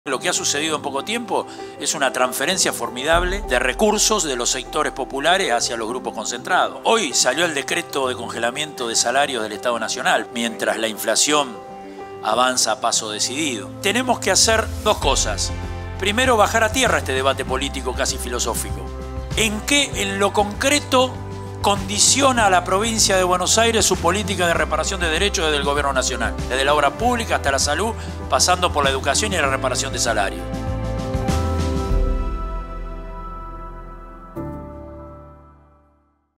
Reunión Multisectorial - Oscar de Isasi ATE
Reunión Multisectorial en La Plata, 5 de febrero.